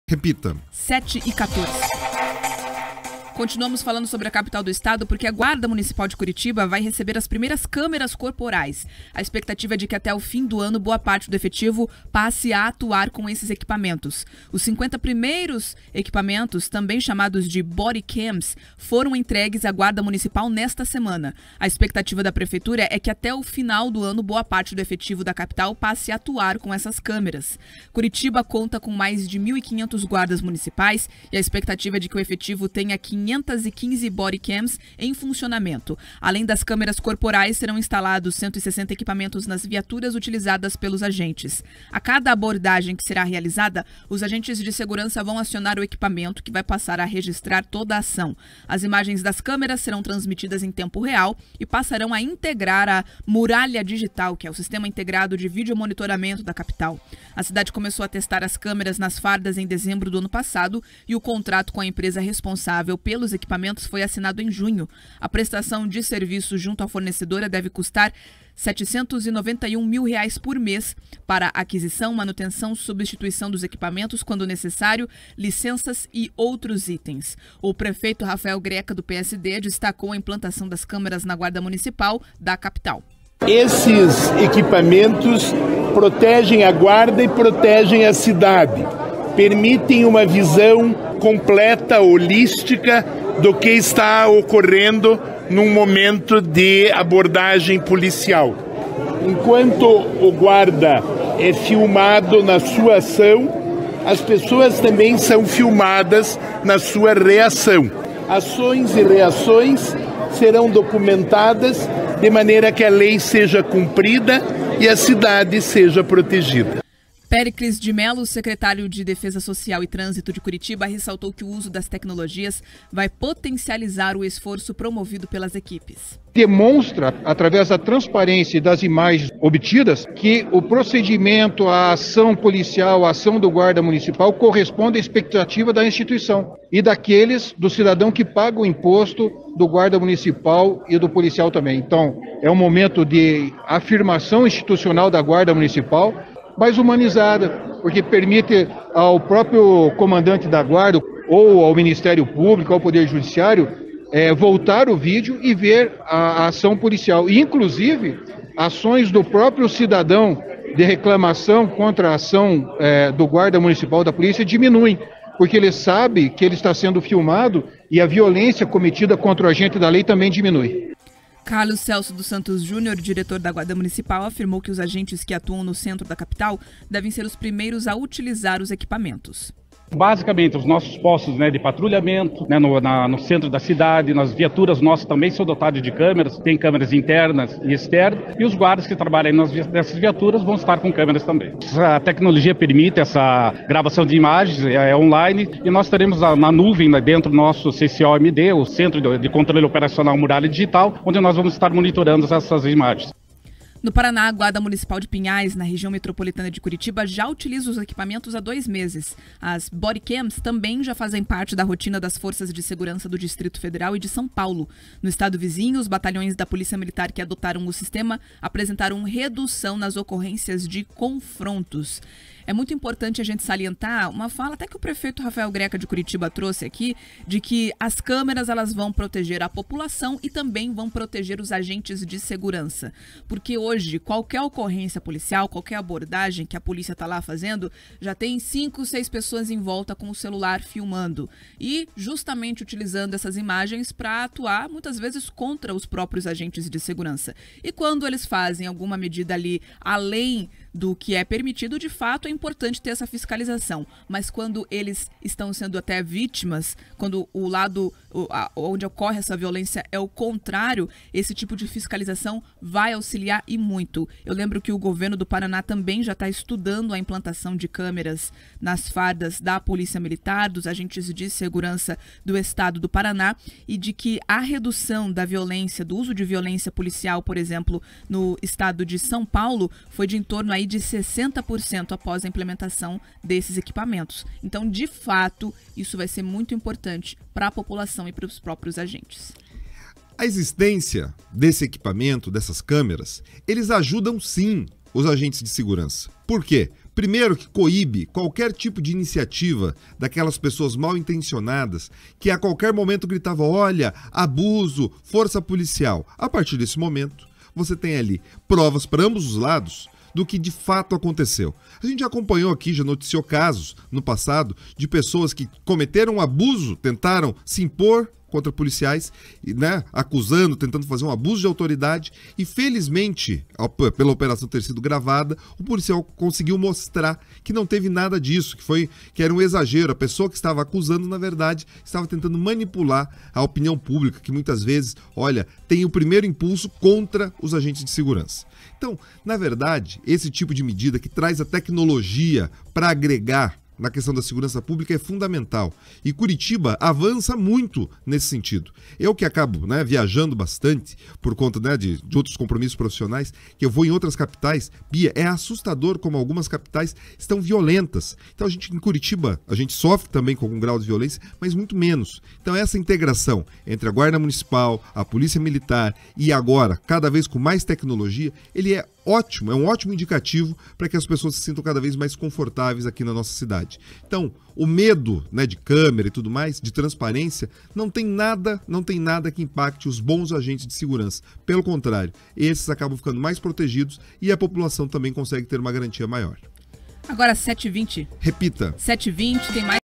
Guardas Municipais com câmeras nas fardas
A Guarda Municipal de Curitiba recebe as primeiras câmeras corporais. A expectativa é de que até o final do ano boa parte do efetivo passe a atuar com os equipamentos. Confira todos os detalhes!